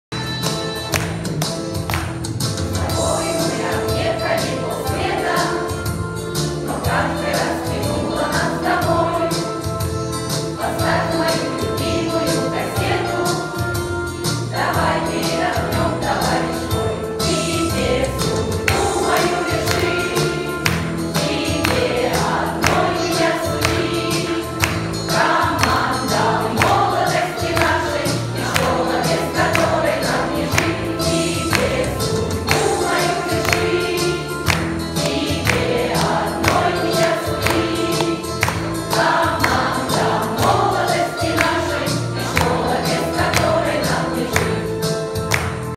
Финальная поздравительная песня от педагогического состава школы
Учителя  поздравляют свою родную сосновоборскую школу №3 с 35-летним юбилеем!